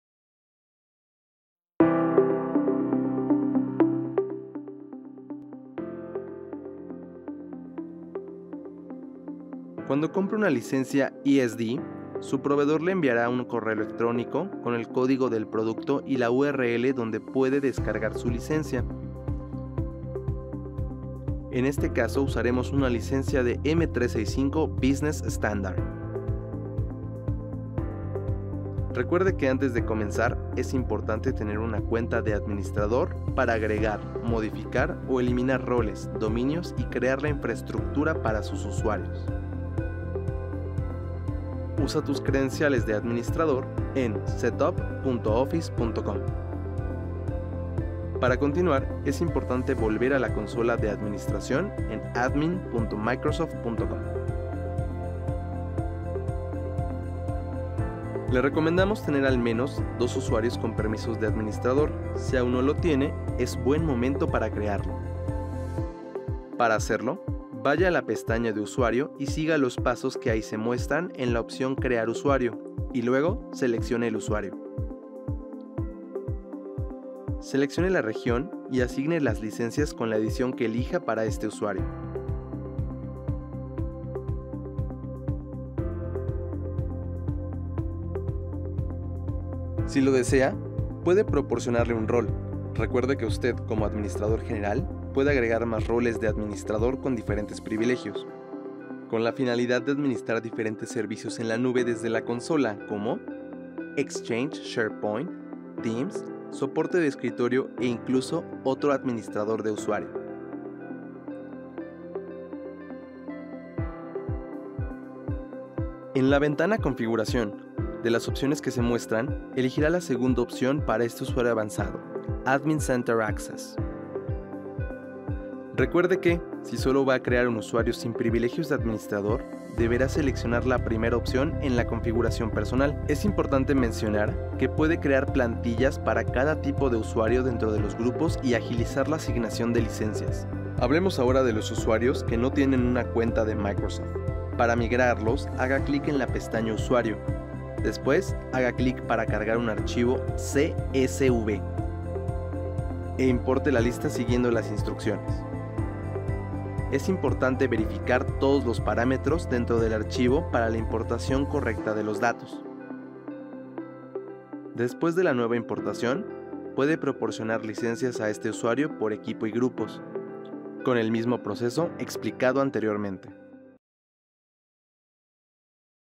Guía para Microsoft 365 Empresa Estándar I Crear roles en el Centro de Administración
Conoce los roles dentro del centro administrativo y limite a qué puede acceder cada usuario.